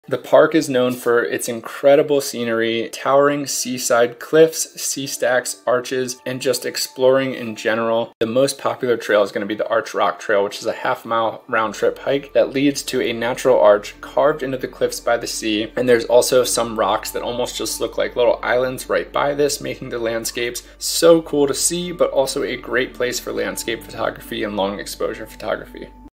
📍Samuel Boardman Scenic Corridor, Oregon
One of the most beautiful scenic drives I have ever done with so much to do. My favorite place here is Natural Bridge, a beautiful rock formation on the coast.

Featuring: Bri Amato @breezydaysahead